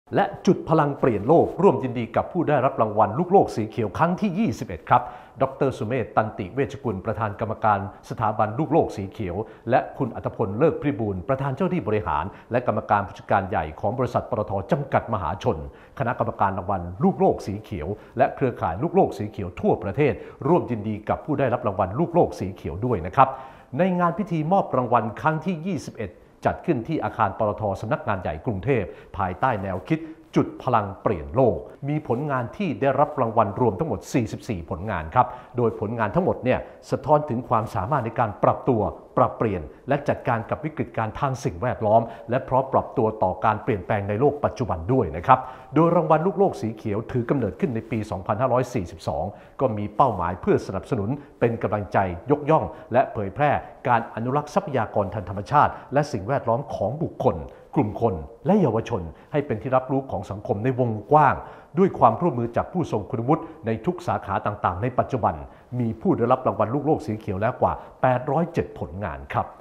ปตท. ร่วมยินดีกับผู้ได้รับรางวัลลูกโลกสีเขียวครั้งที่ 21
ปตท. ร่วมยินดีกับ 44 ผลงานทรงคุณค่าด้านการดูแลสิ่งแวดล้อม ที่ได้รับรางวัลลูกโลกสีเขียว ครั้งที่ 21 ภายใต้แนวคิด “จุดพลังเปลี่ยนโลก”